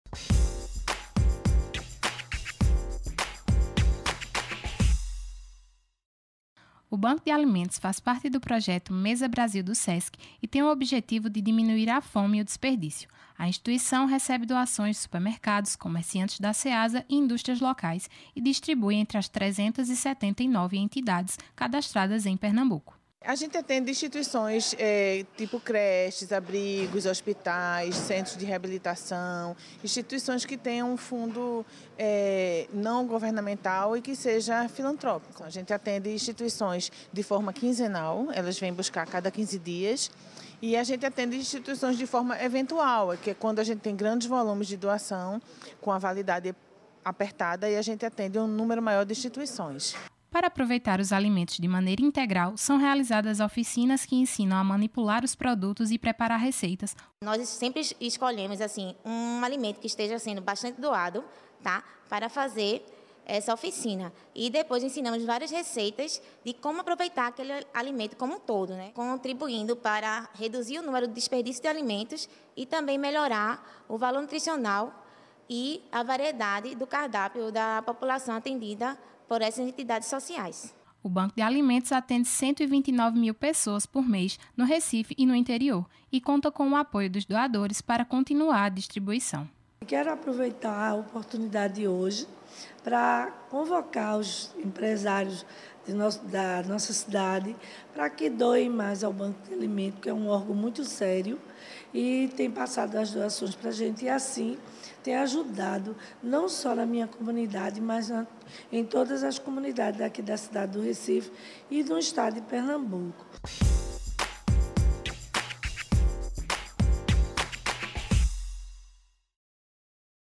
Alimentação saudável
O Banco de Alimentos é um projeto do Mesa Brasil SESC que tem como objetivo reduzir a fome e o desperdício de alimentos em Pernambuco. Por meio de doações feitas por supermercados, comerciantes da Ceasa e pela indústria local, 379 instituições são beneficiadas. São escolas, abrigos, creches e hospitais que podem contar quinzenalmente com uma alimentação saudável e equilibrada.